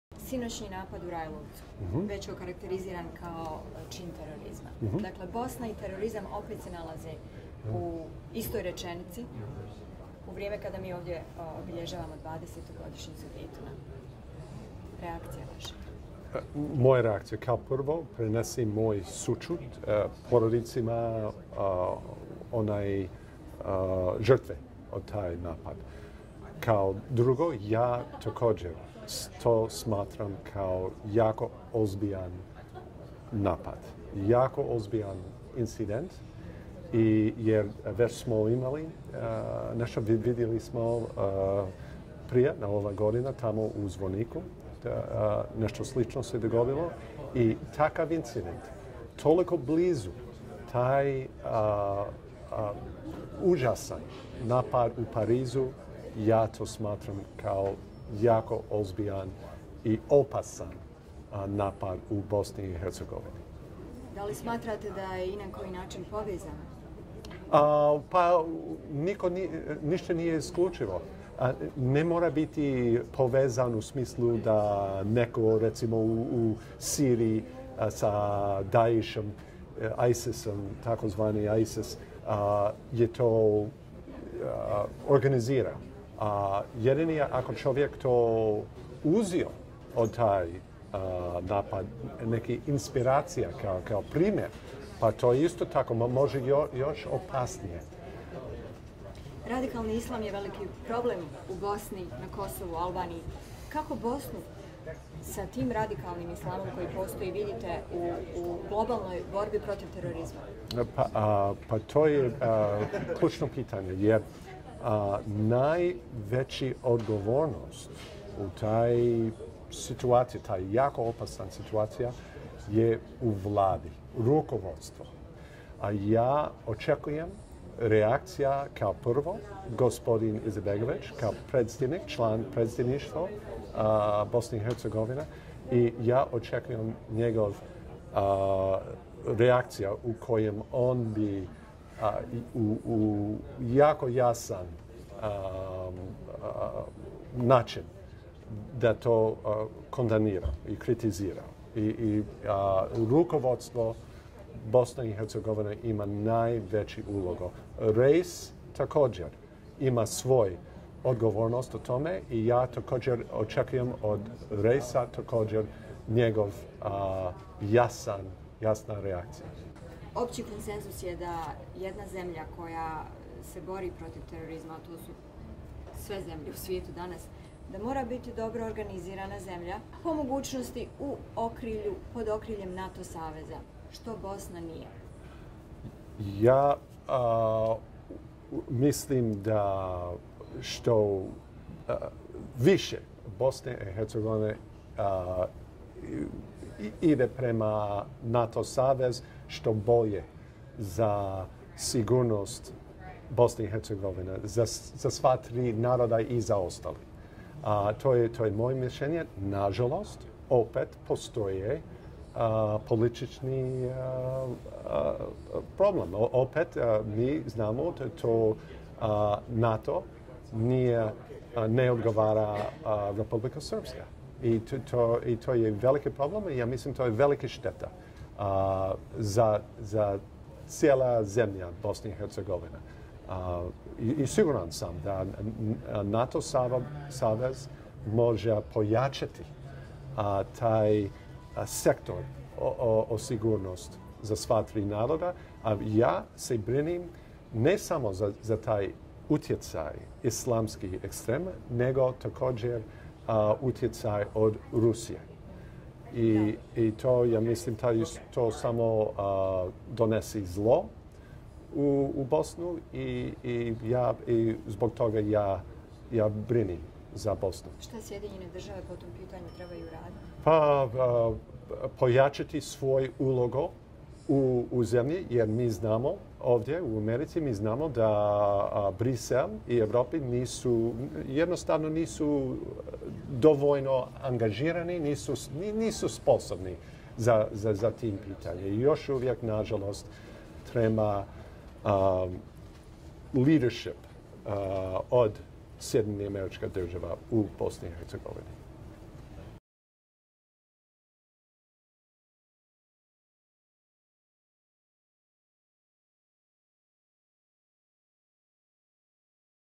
Edward Joseph: ''Potrebno da se oglasi Reis BiH''
Stručnjak iz Centra za transatlanske odnose i dobar poznavalac Bosne komentira sinoćnji napad na Rajlovac.